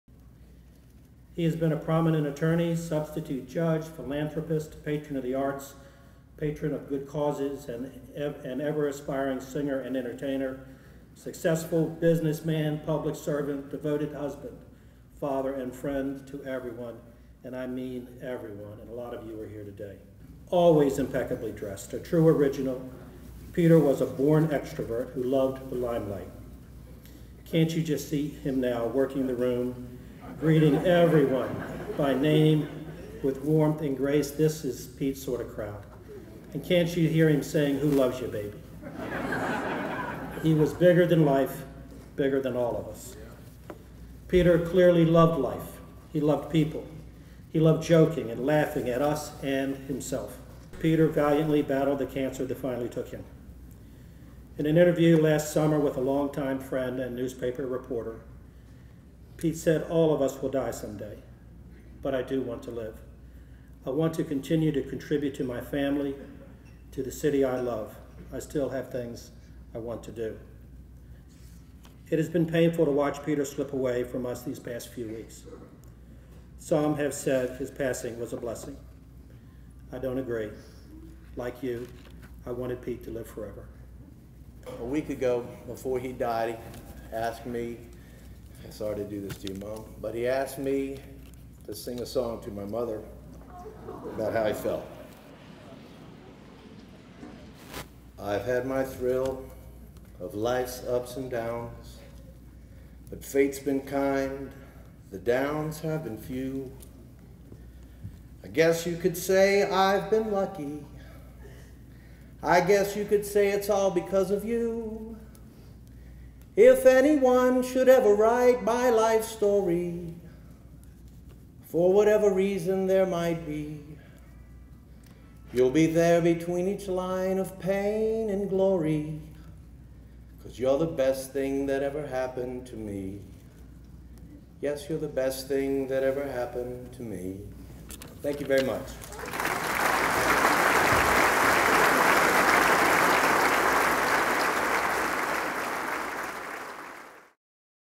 Thousands flock to Norfolk service for Peter Decker Jr.
Scenes from the public memorial service of Peter Decker Jr.